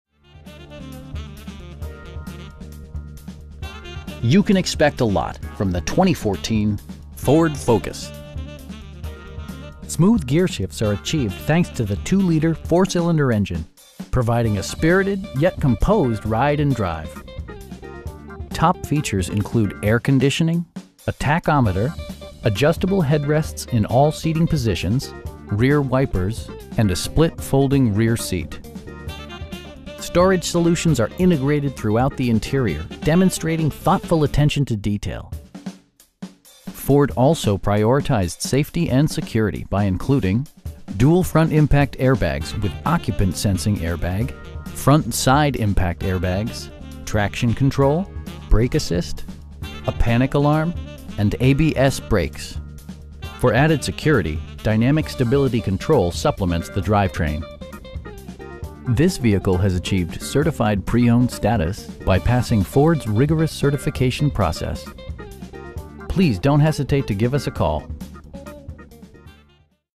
2014 Ford Focus SE in Lancaster, CA 93534
You can expect a lot from the 2014 Ford Focus. Smooth gearshifts are achieved thanks to the 2 liter 4 cylinder engine, providing a spirited, yet composed ride and drive. Top features include air conditioning, a tachometer, adjustable headrests in all seating positions, telescoping steering wheel, power door mirrors, cruise control, rear wipers, and a split folding rear seat. Storage solutions are integrated throughout the interior, demonstrating thoughtful attention to detail. Ford also prioritized safety and security by including: dual front impact airbags with occupant sensing airbag, front side impact airbags, traction control, brake assist, a panic alarm, and ABS brakes. For added security, dynamic Stability Control supplements the drivetrain. This vehicle has achieved Certified Pre-Owned status, by passing Ford's rigorous certification process. You will have a pleasant shopping experience that is fun, informative, and never high pressured. Please don't hesitate to give us a call.